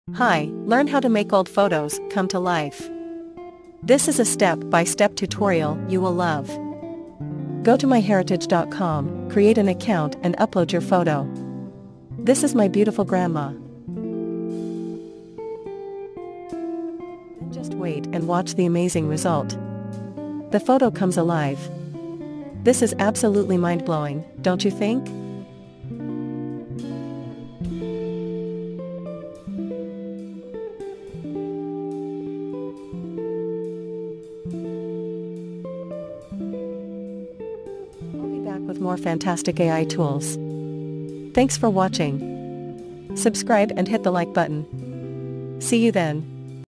TechForYou- Make Photos Come To Life
You will easily learn how to make videos from old photos using AI for free in this step by step tutorial.   Let's create amazing projects together.